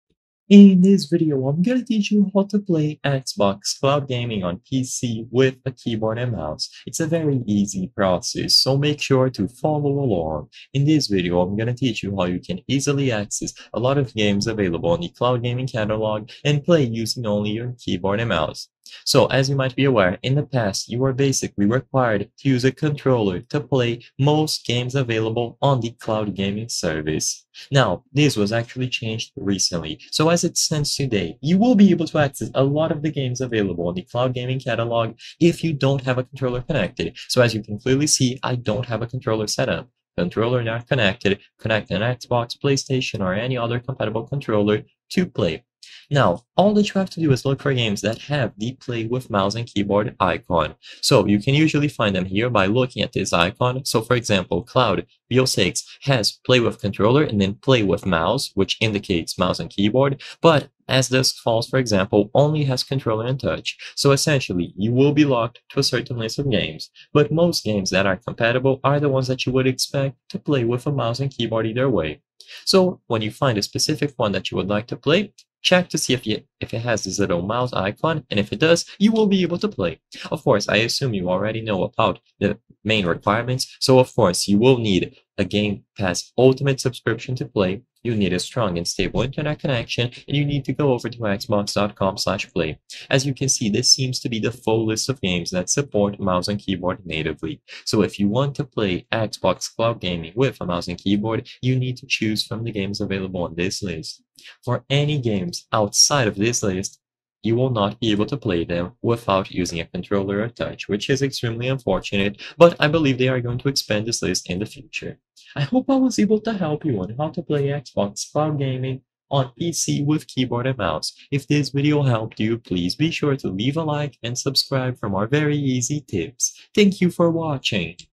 How To Play XBOX Cloud Gaming on PC with Keyboard and Mouse (2025) - Step by Step
I will solve your doubts about how to play xbox cloud gaming on pc with keyboard and mouse, and whether or not it is possible to do this.

I hope I have helped you with the video!